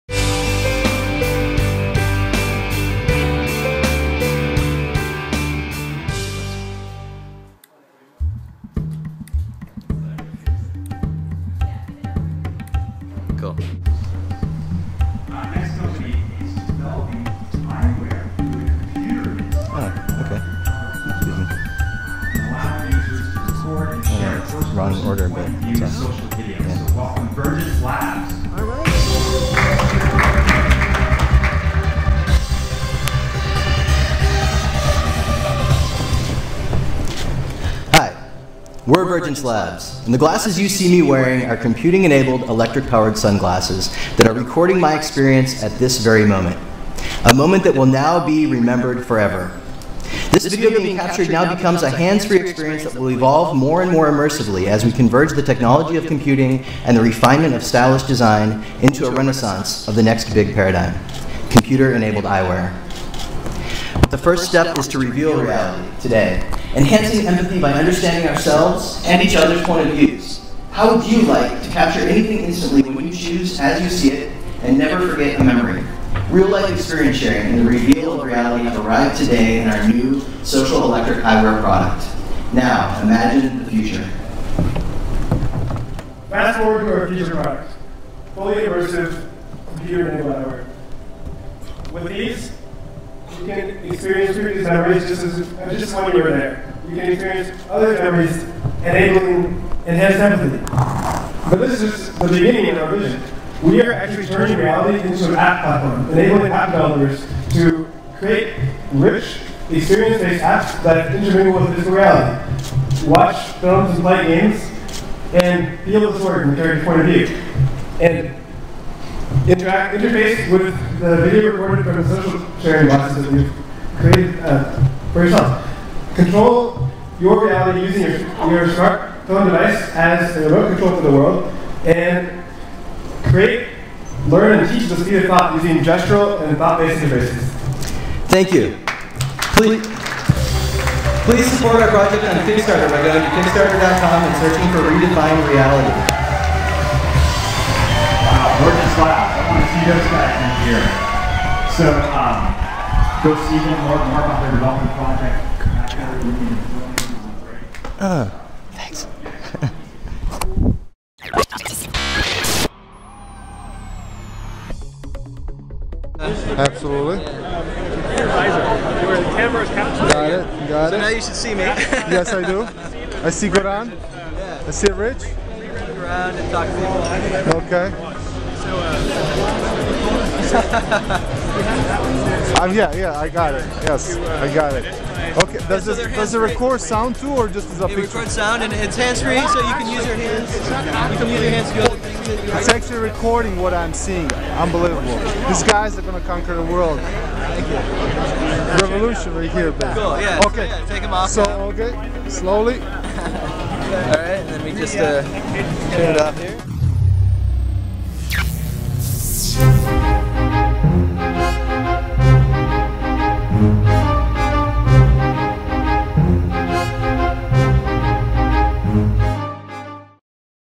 Vergence Labs debuts Epiphany Eyewear at DEMO conference 2012
Vergence Labs had the privilege of presenting on the main stage at the infamous DEMO conference 2012, the same conference where e*trade, VM Ware and Tivo, amongst many others, launched their products. It was incredible to debut our alpha pitch to such a captive audience comprised of industry and press veterans of such a wide breadth and scope in the PC industry.  Thank you Microsoft for the full sponsorship of the alpha pitch program and for supporting Vergence Labs!